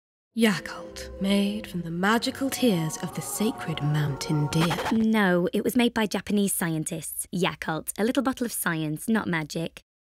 Yakult | Science Not Magic | Deer 10"
At Yakult our scientists, led by Dr Shirota cultivated and selected a unique strain of bacteria that is scientifically proven to reach the gut alive, and this goes into every bottle. No nonsense. Just billions of bacteria.